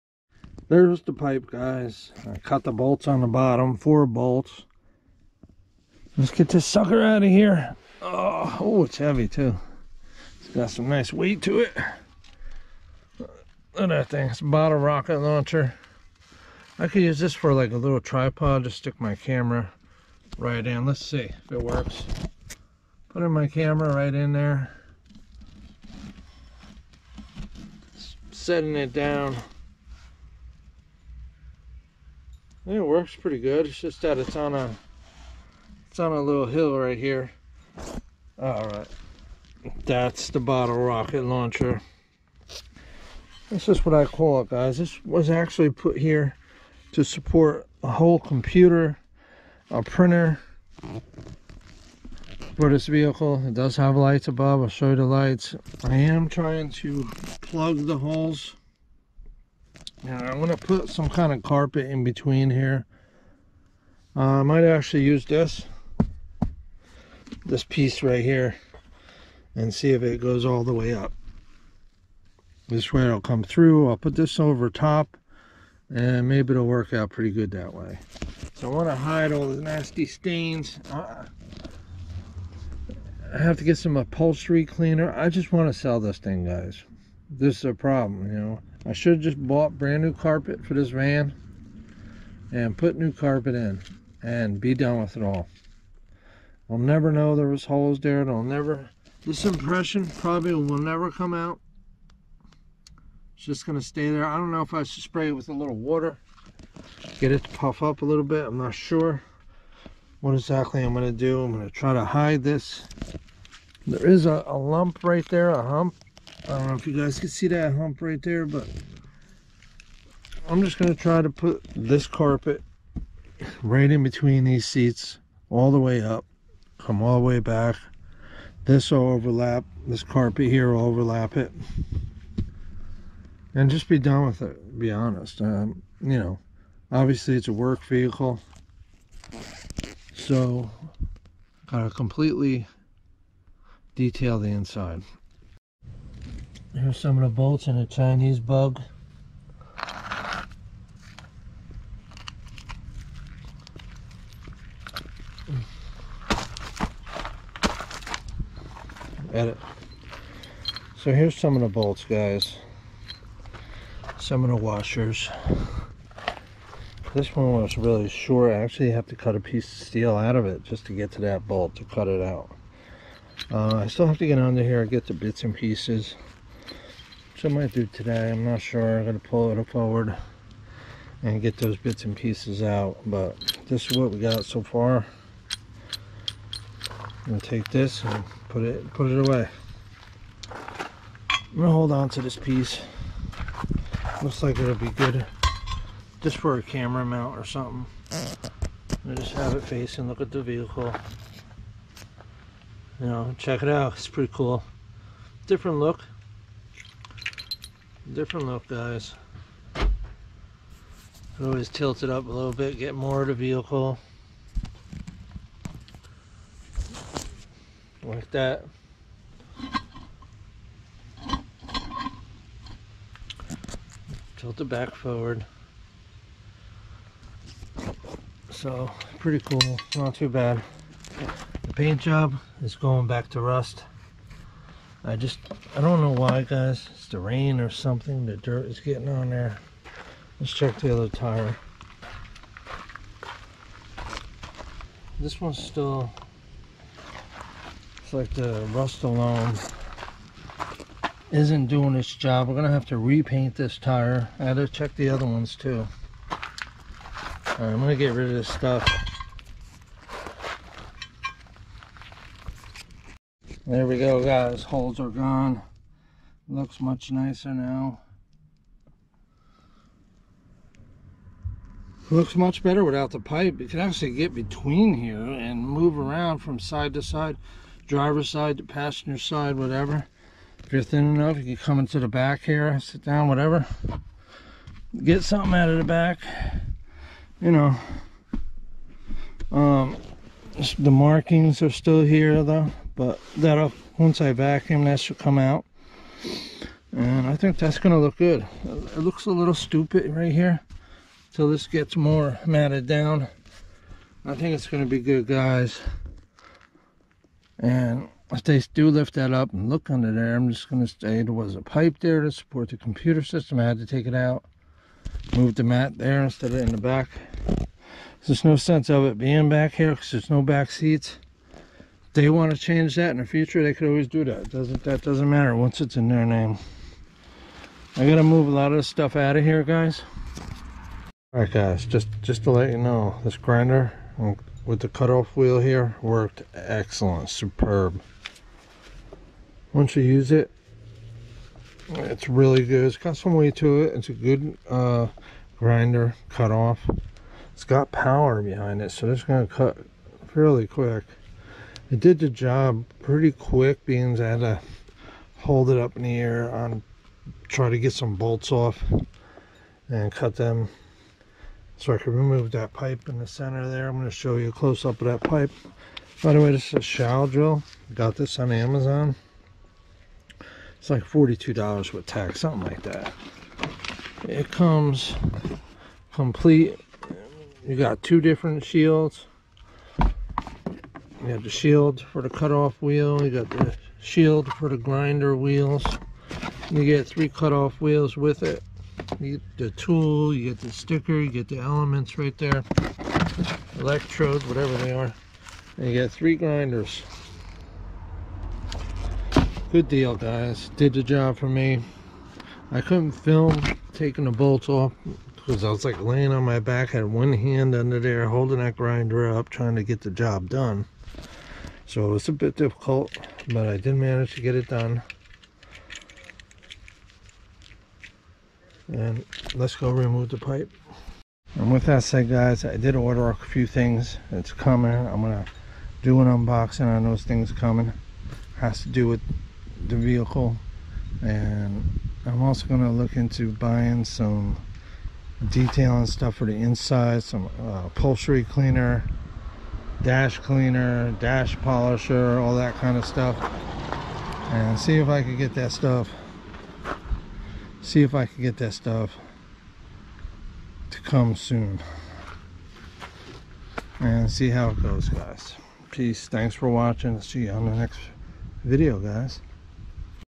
Making vehicle look great then for sale
removed the pipe between the front seats, I used the shall,it worked great